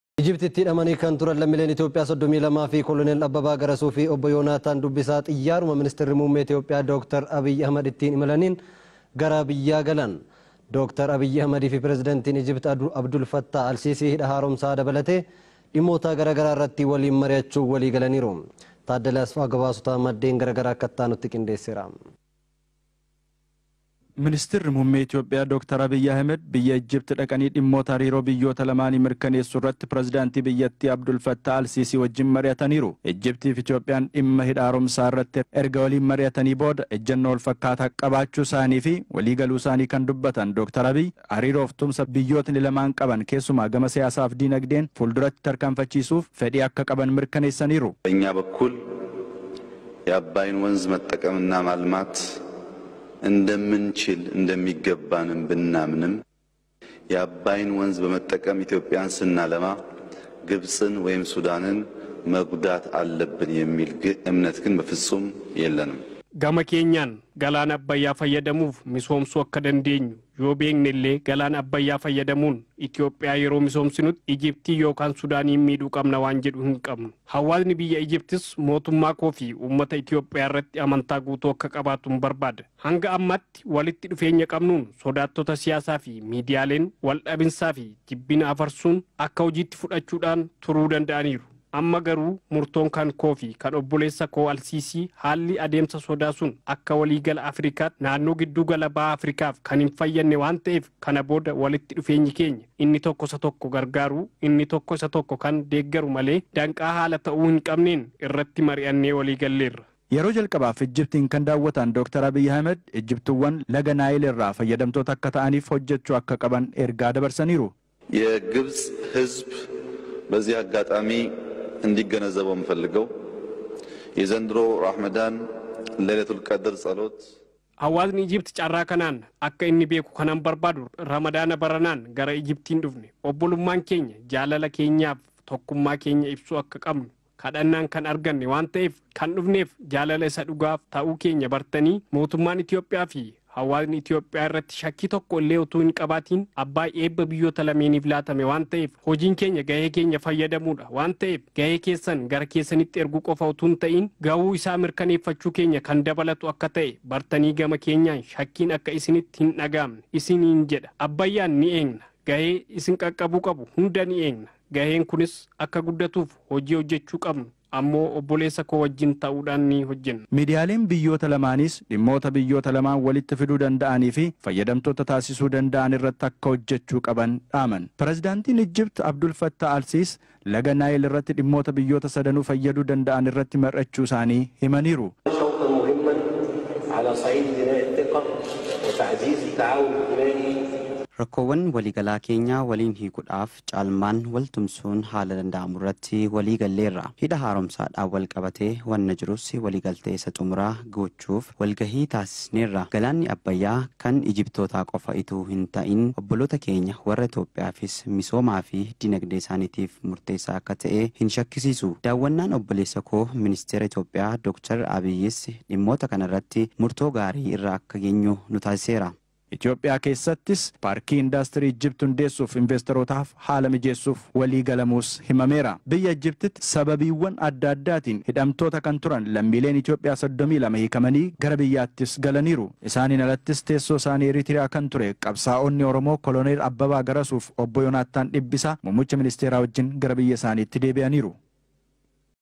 TOP NEWS Lammiilen Itiyoophiyaa Ijiptii hidhaarra turan 32 hiikaman
Lammiilen Itiyoophiyaa Ijiptii hidhaarra turan 32 hiikaman